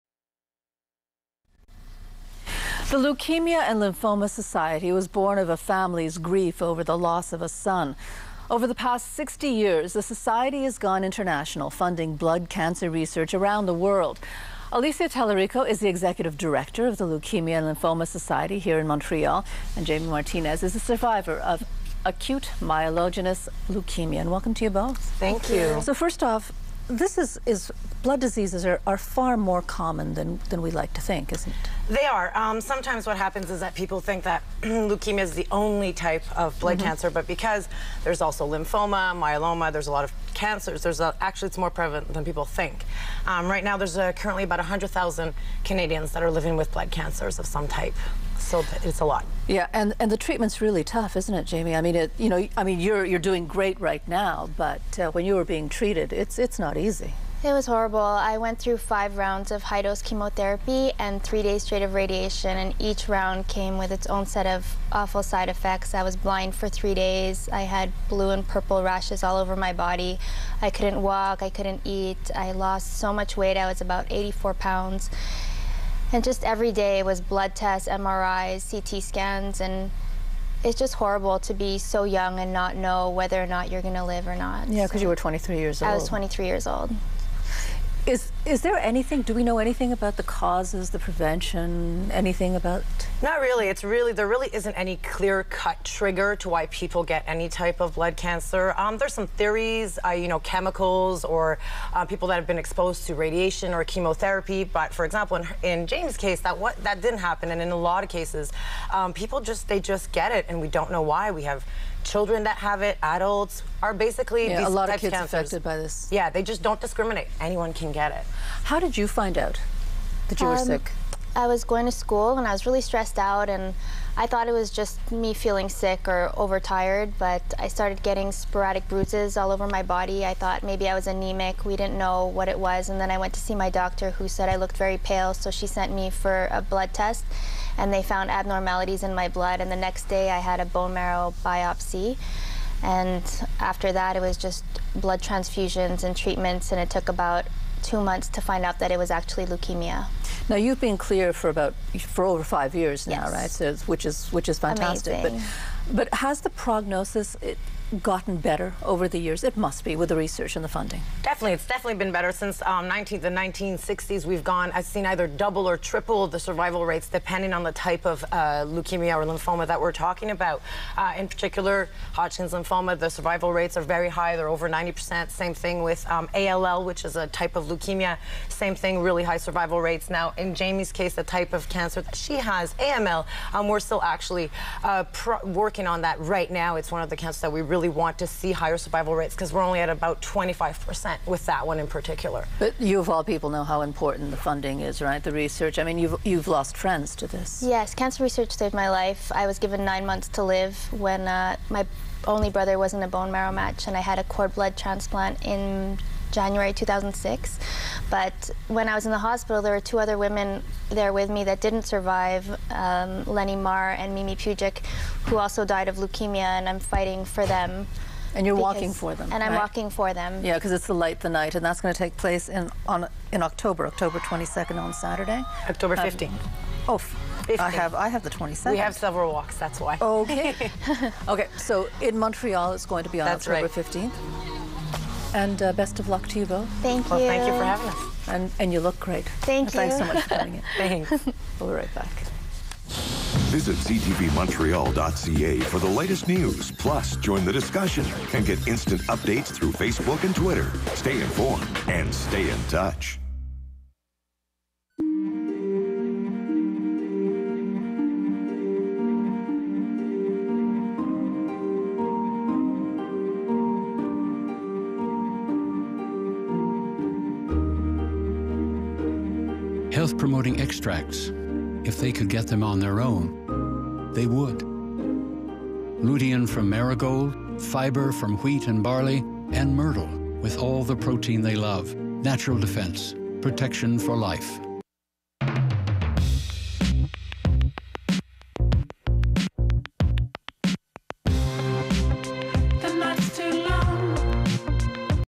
CTV Montreal- Jaymie Martinez Lights The Night
AML survivor, Jaymie Martinez speaks about her diagnosis, treatment and why she is walking at the October 15th Light The Night Walk in Montreal, benefiting The Leukemia & Lymphoma Society of Canada.